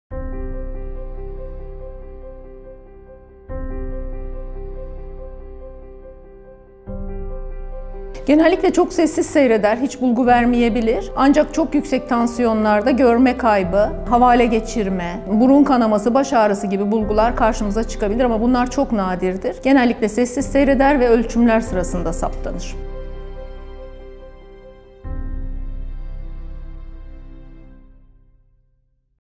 Çocukta Hipertansiyon Belirtileri Nelerdir?
Prof. Dr. Zelal EKİNCİ, çocukta hipertansiyon belirtileri hakkında bilgi verdi.